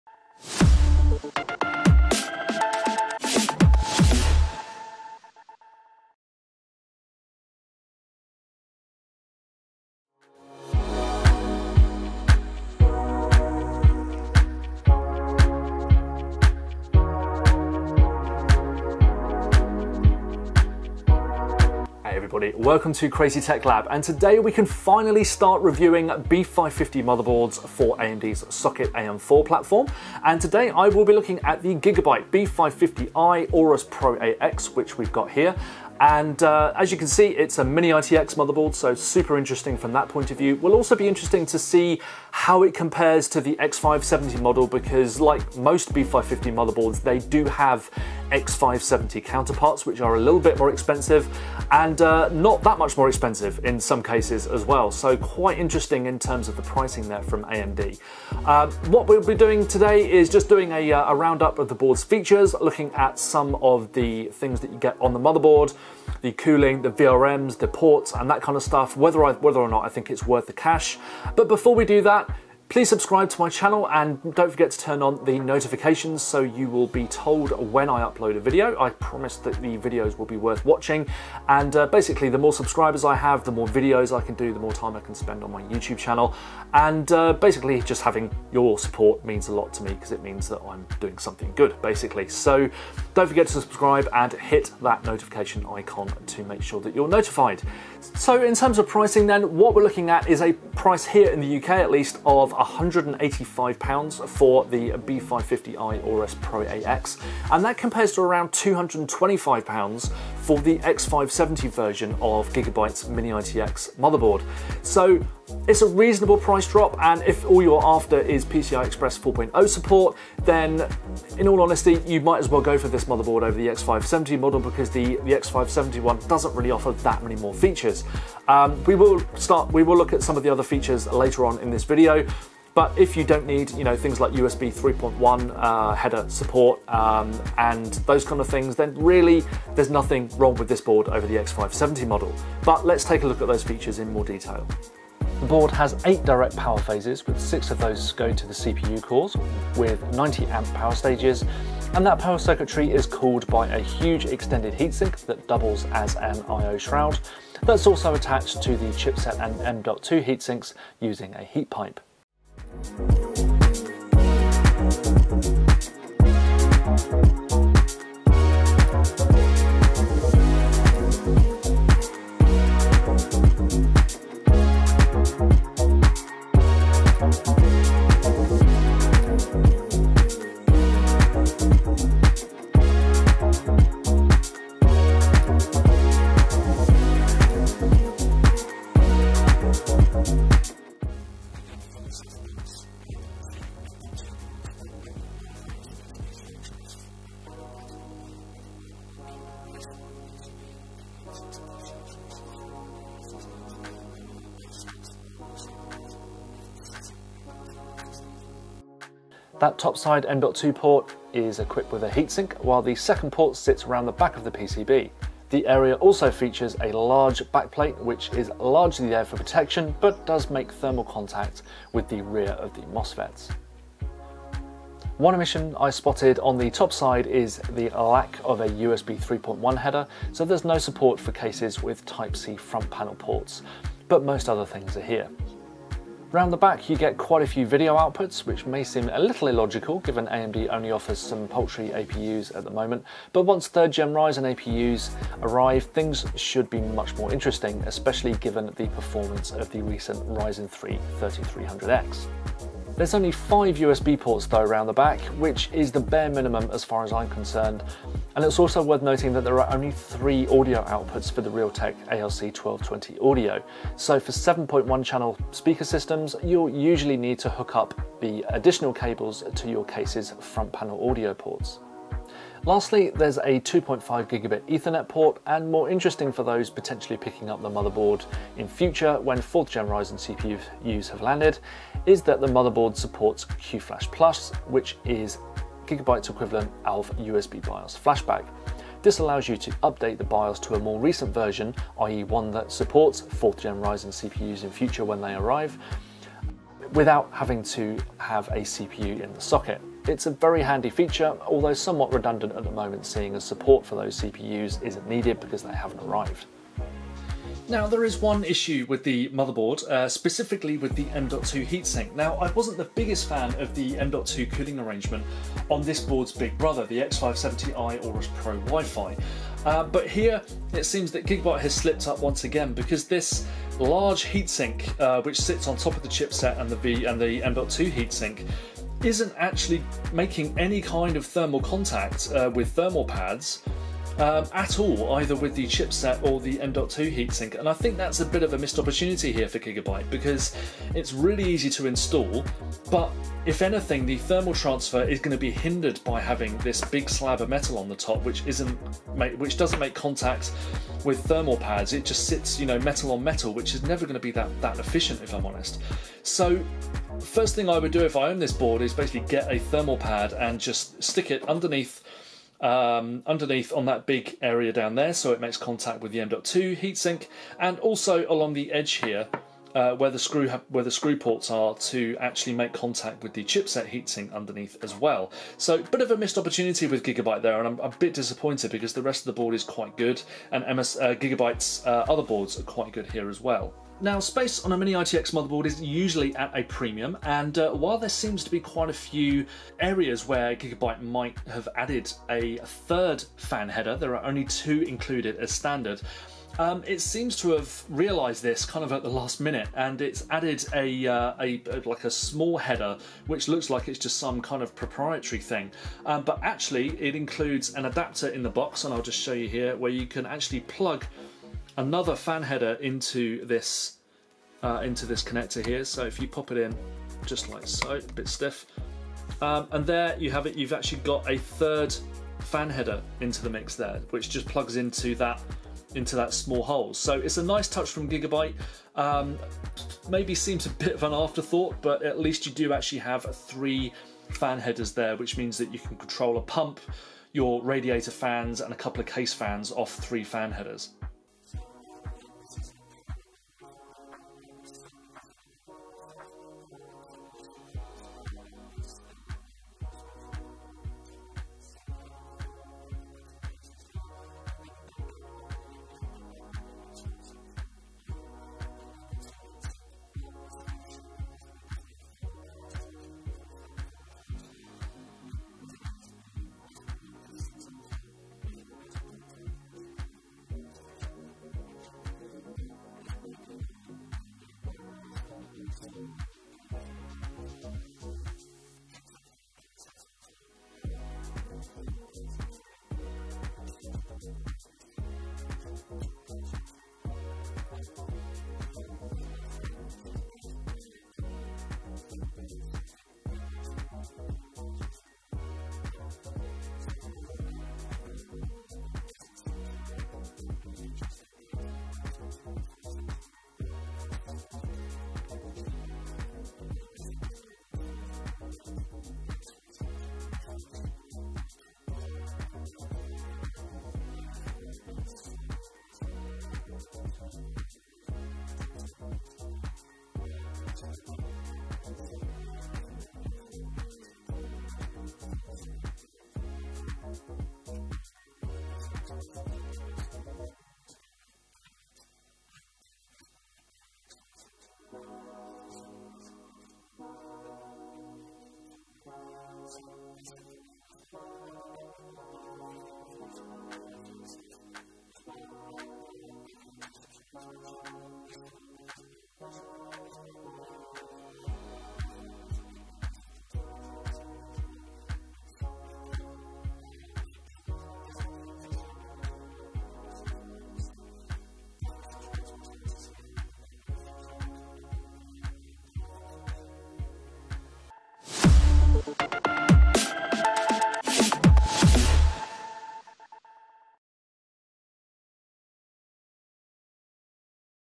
Gigabyte B550I Aorus Pro AX - THE BEST AM4 MINI-ITX?
An in-depth look at Gigabyte new mini-ITX board for Ryzen CPUs - the B550I Aorus Pro AX, which uses AMD's new B550 chipset